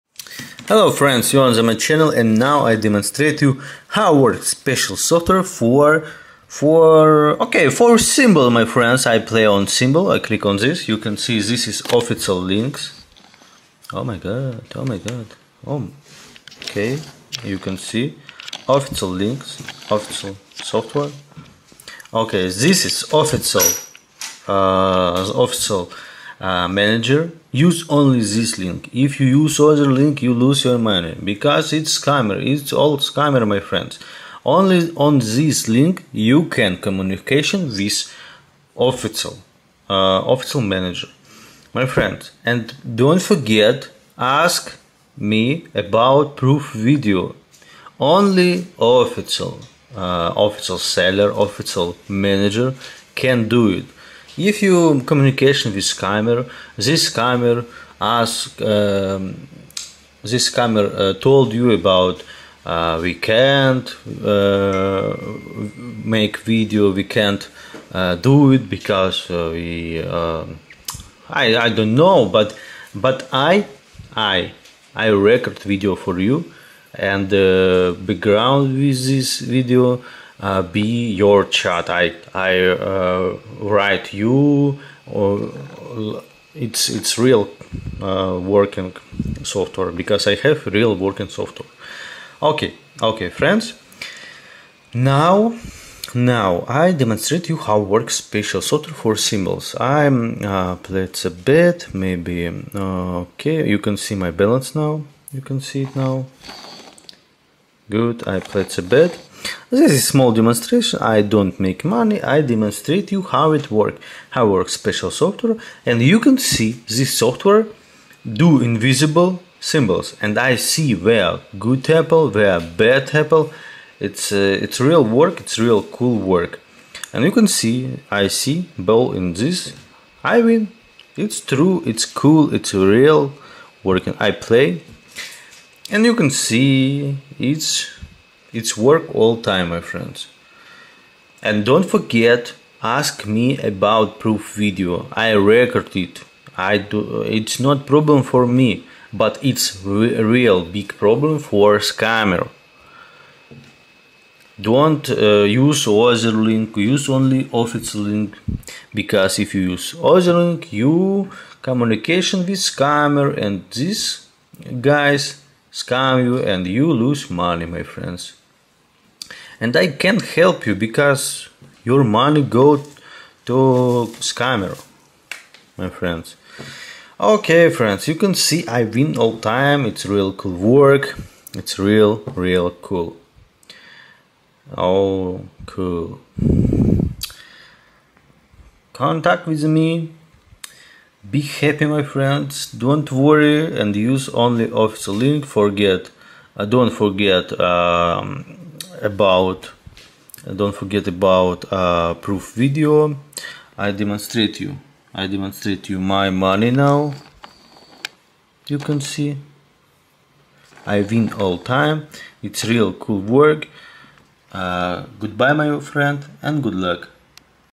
Gagnez tous les jours avec l'aide de assistant de script thimbles sur la plate-forme 1xBET
Envoyez-nous un message dans un telegram pour tout problème qui se pose. Programme thimbles améliorer automatically. 1xBet plaide auprès de notre communauté ne plus jouer. Regardez la courte vidéo jusqu'à la fin pour savoir comment cela fonctionne. Alors rencontre nouvelle version programme thimbles. Notre brigade inlassablement mise à jour et développement cette triche.

url de mon compte telegram pour la communication